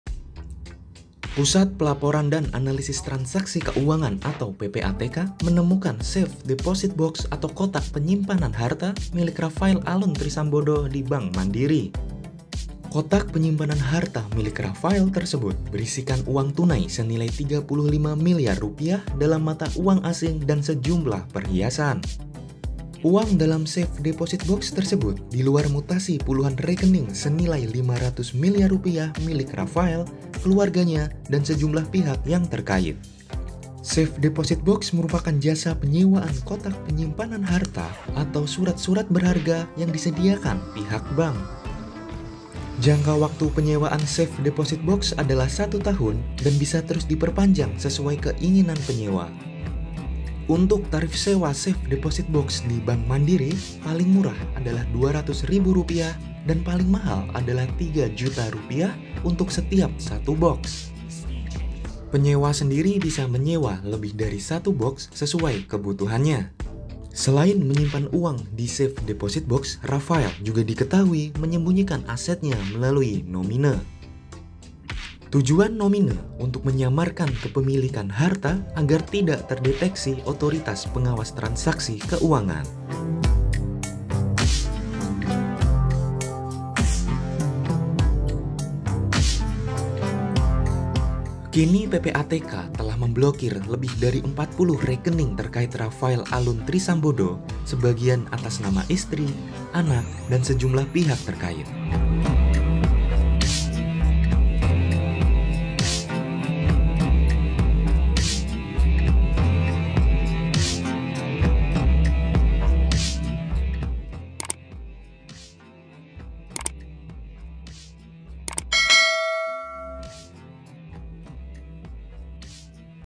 Safe Deposit Box Rafael Berisi Rp 37 Miliar hingga Perhiasan
Pusat Pelaporan dan Analisis Transaksi Keuangan (PPATK) menemukan safe deposit box milik Rafael Alun Trisambodo di Bank Mandiri. Kota penyimpanan harta milik Rafael tersebut berisikan uang tunai senilai Rp 35 miliar dalam mata uang asing dan sejumlah perhiasan.

Menurut PPATK uang dalam safe deposit box tersebut di luar mutasi puluhan rekening senilai Rp 500 miliar milik Rafael, keluarganya, dan sejumlah pihak yang terkait. Sampai saat ini safe deposit box tersebut masih dalam kewenangan PPATK dengan status blokir untuk proses analisis lebih lanjut.

Video Jurnalis: TAL, ARI, Wisang
Penulis: Muhammad Idris
Penulis Naskah: Dica Septhian Herlambang
Narator:Dica Septhian Herlambang
Video Editor: Dica Septhian Herlambang
Produser: Elizabeth Prillia Yahya Carvallo